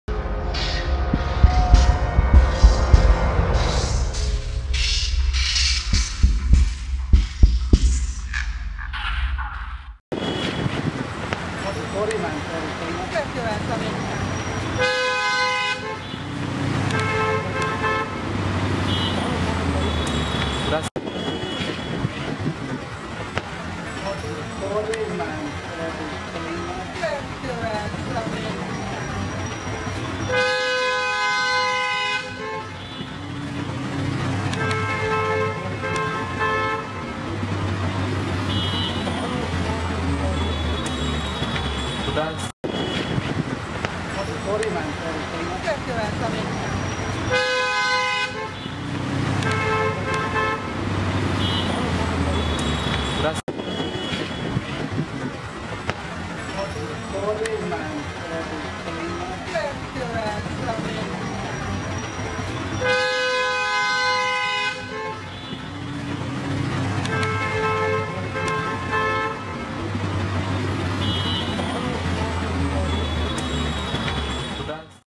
WHAT! Dimple Kapadia Spotted In Auto Rickshaw In Juhu | DIMPLE Travels In Rickshaw
WHAT! Dimple Kapadia Spotted In Auto Rikshaw In Juhu  | DIMPLE Travels In Rickshaw